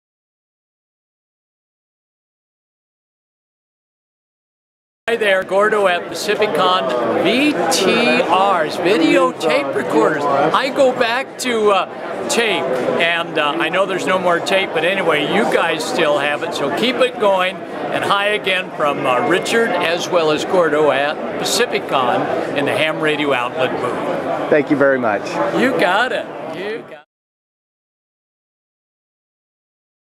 Gordon West sends greetings to the OLDVTRS forum
Gordo was gracious enough to humor me and send his best wishes to the members of our discussion forum on Yahoo Groups. The OLDVTRs forum is the place to discuss that rusty old video recorder that drives your curiosity.